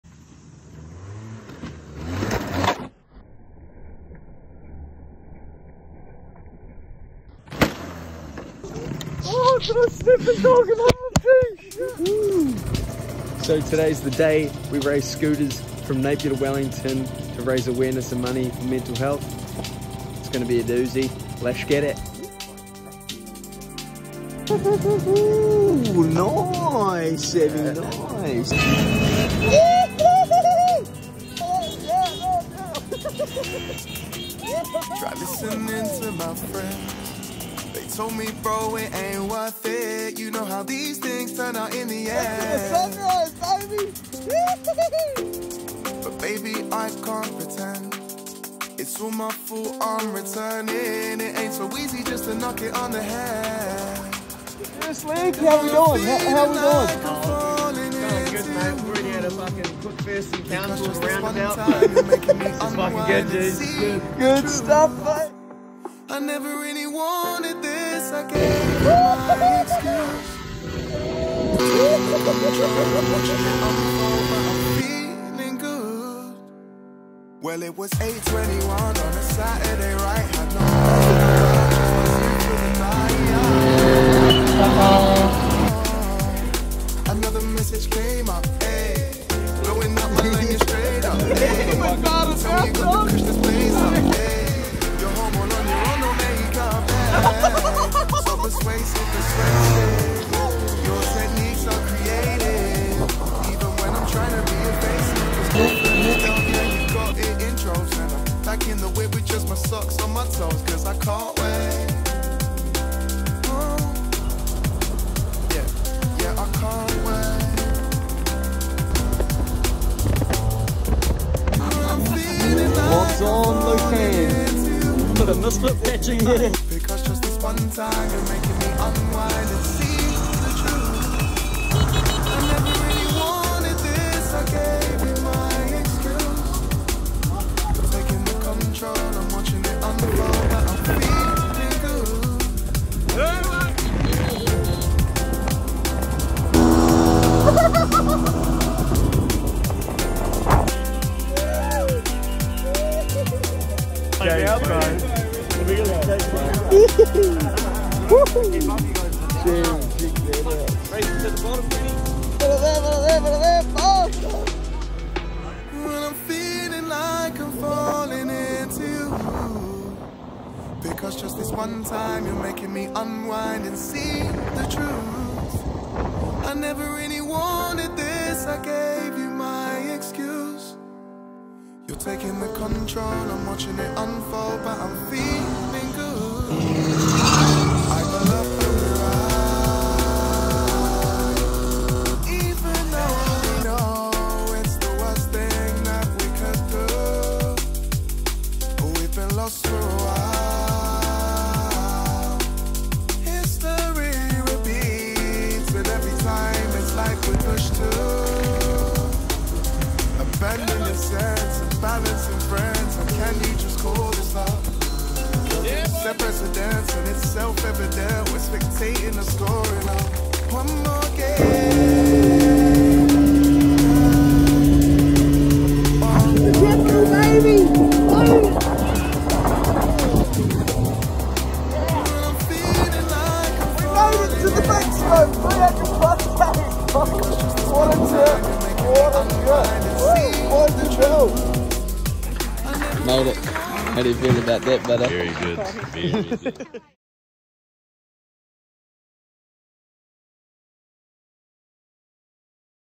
2022 NAPIER TO WELLINGTON MOPED RACE FUNDRAISER
Here it is the highly anticipated after movie of the MSFT Moped crew’s mission from Napier to Wellington travelling over 320km on the 50cc rigs. For the purpose of raising awareness/action for mental health aswell as raising a current $77,000+ for @awhitrust & @iamhope_nz (incredible).

Nothing but laughs & good times from start to finish with the crew and everyone else that we crossed paths with on the day. Massive ups to all 53 riders that got amongst it & the legendary organisers that made it all happen, we were stoked to be apart of the first year of the event & no doubt many to come!

Follow MSFT Productions Social channels to keep up with our daily missions around the country side -